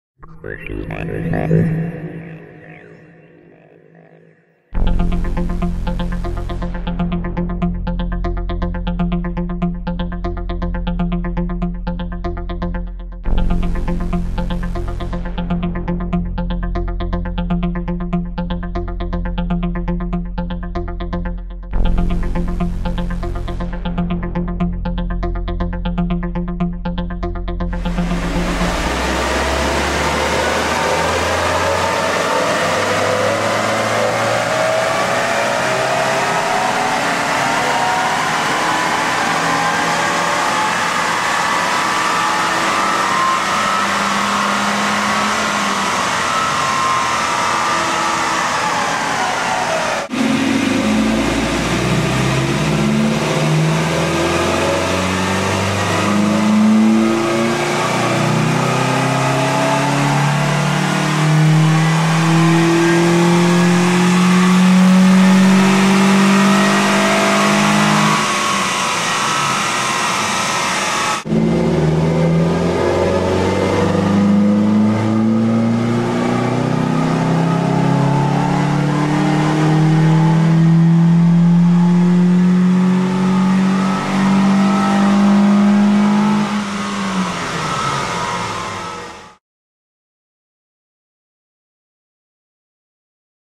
Takeda Cold Air Intake - 2008 Scion xD Dyno.flv
This Scion xD gets dynoed with a Takeda Cold Air Intake and no other engine mods.

Find out how much power it gains by selecting your vehicle